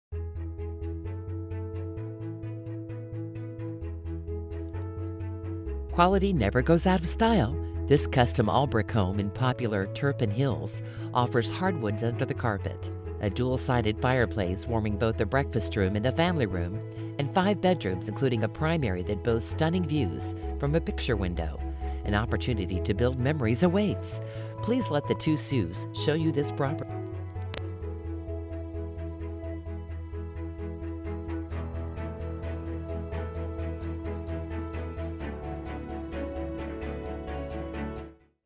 5813 Lengwood Dr Anderson Twp, OH | MLS# 1776248
5813 Lengwood Dr, Anderson Twp, OH

Quality NEVER goes out of style! One owner, custom, all brick, 5 bdrm 4.5 bath home in popular Turpin Hills. Fabulous view of backyard from picture window in primary bedroom. Double sided fireplace warms both breakfast room and family room. Hardwood protected by carpet. An opportunity to build memories awaits! Schedule a private showing today!